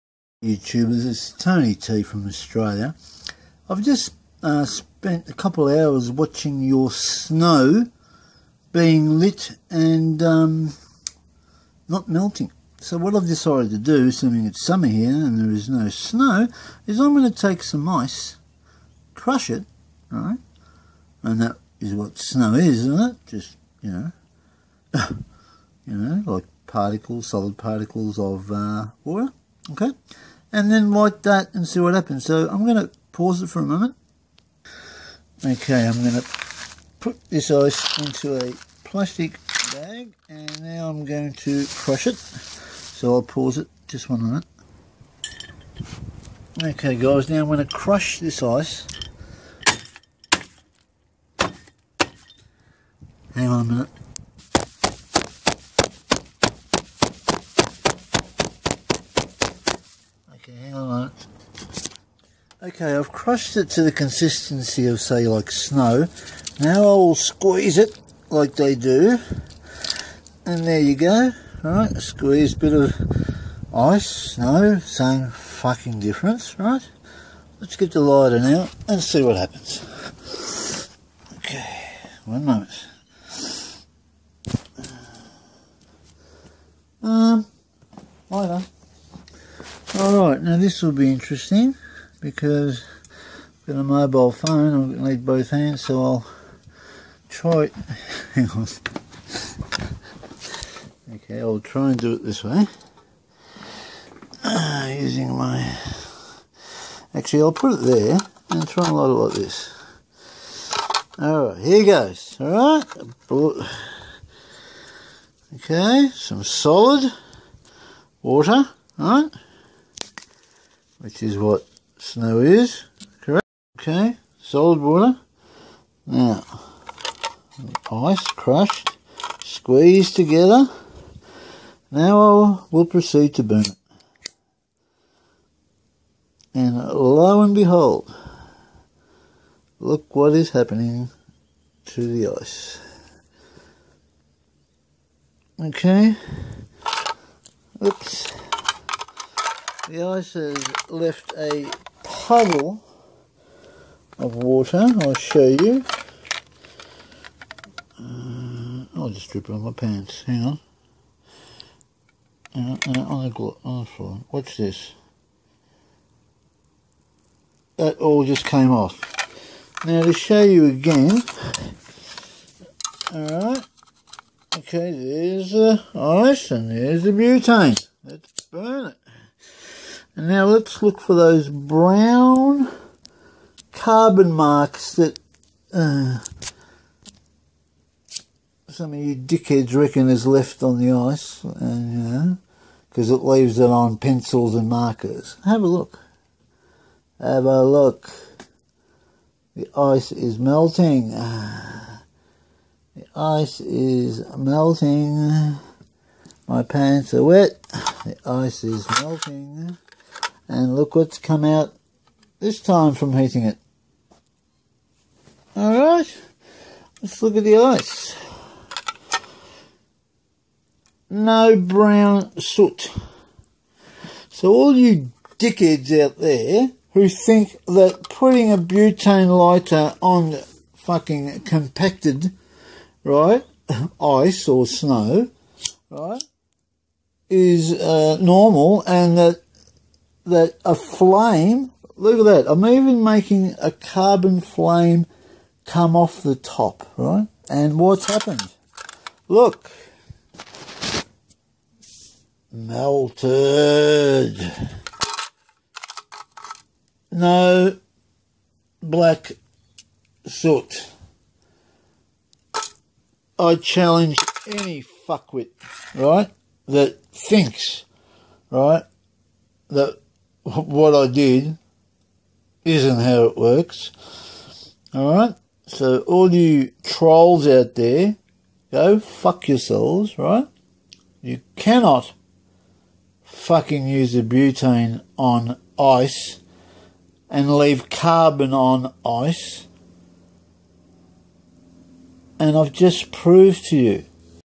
Proof Ice won't go black and melts with butane!
I tried to mimic using a butane lighter on ice made from water and show no soot, no carbon and it melts.
If you think you debunked this you are full of shit and a troll
You cannot burn water Dicks!